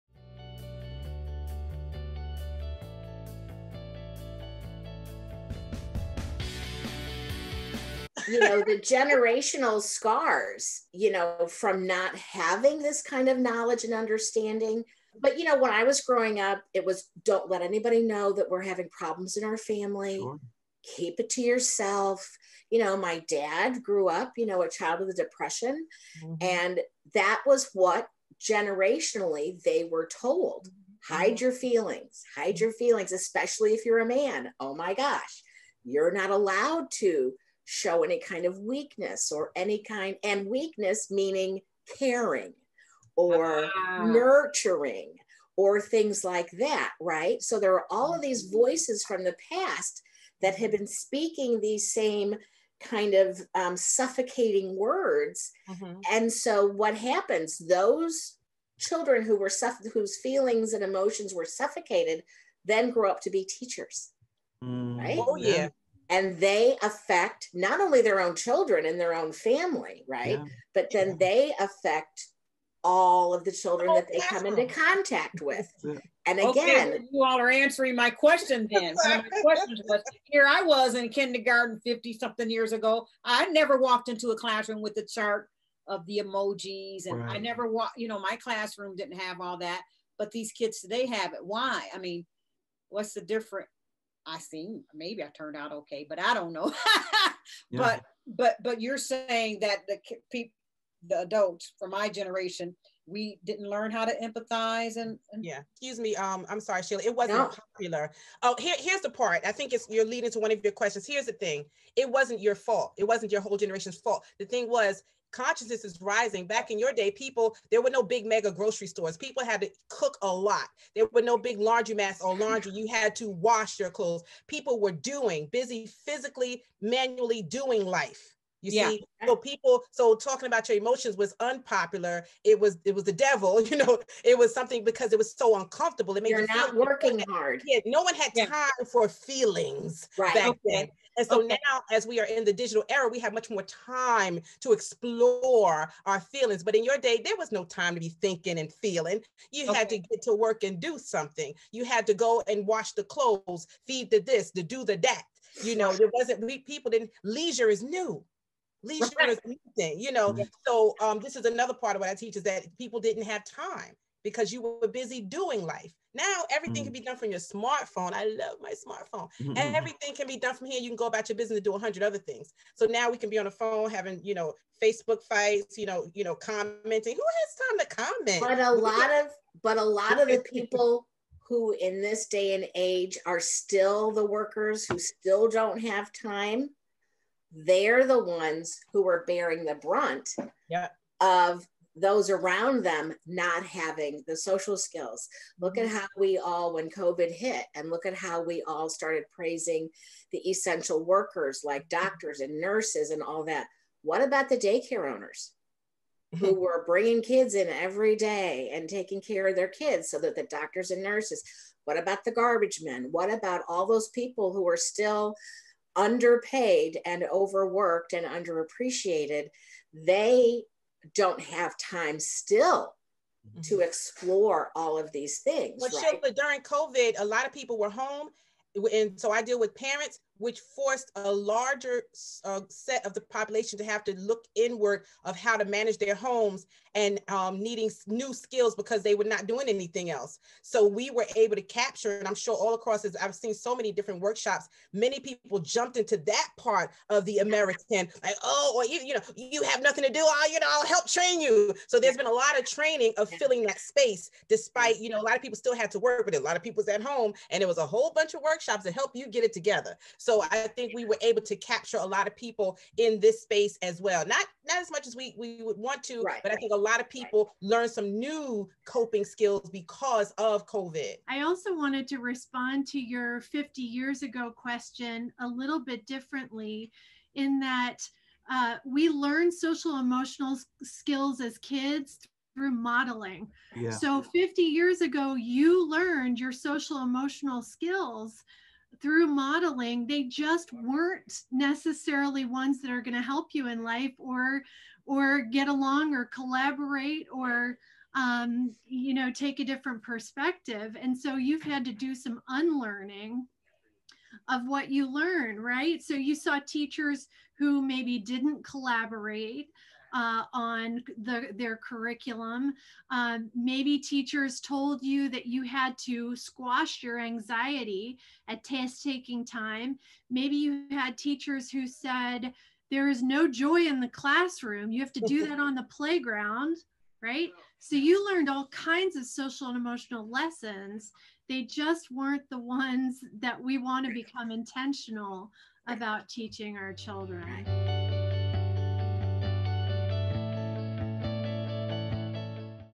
Part Four--Baby Boomers Didn't Learn SEL. Why Are Kids Learning It Now? A Panel Discussion
In part four of this series, four social and emotional (SEL) experts discuss the effect of SEL on the Baby Boomer generation and today's society.
What is this series about?
This discussion sprang from the publication of.the book How Are You Today? A Celebration of Children's Emotions by Linda M. Mitchell. Use your words! That’s what parents and teachers often say to children. Those "emotion words" are a hot topic in education, often referred to as social and emotional learning or SEL. Research shows that one of the greatest predictors of a child's success in school is their social and emotional health. This book is a game-changer for educators, counselors and parents to use in their efforts to teach kids to name their feelings. The panelists will establish that SEL is a much needed tool to help not only kids but also for adults become successful, healthy, articulate human beings!

Social and Emotional Learning Panelists
Sheila Oliveri
Early Childhood Literacy and Curriculum Coordinator
Ready Readers
St. Louis, MO

Jennifer Miller
Family and Educational Consultant/Coach, Social and Emotional Learning
Founder, Confident Parents, Confident Kids Blog
Author, Confident Parents, Confident Kids
Columbus, OH

Love M. Gaillard, MSW
Social Emotional Learning Facilitator
West Orange, NJ

Derrick Booth
Director of Social Emotional Learning
Peoria Public Schools
Wraparound Center
Peoria, IL

How Are You Today? A Celebration of Children's Emotions

Thumbnail created on Canva.com and Adobe Photoshop by Linda M. Mitchell

Photography by Odell Mitchell Jr.

Thumbnail created on Canva.com and Adobe Photoshop by Linda M. Mitchell aka Literacy is Liberation!